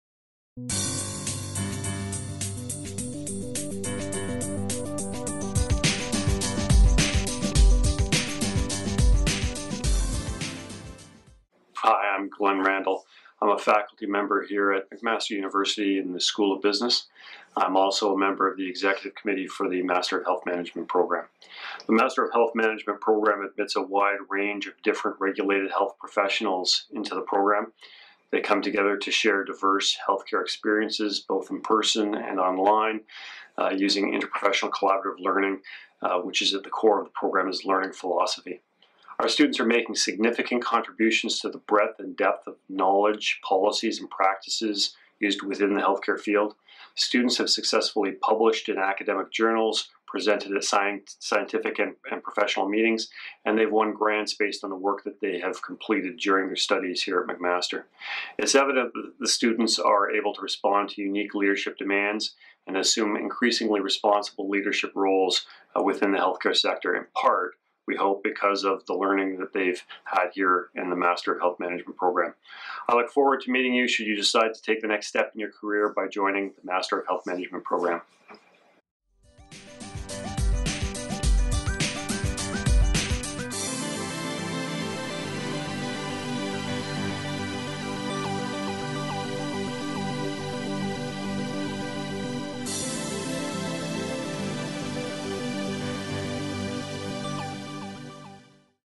The Success of Master of Health Management Students
Glen Randall, a member of the Executive Team for the Master of Health Management program, talks about the sucess of students in the program.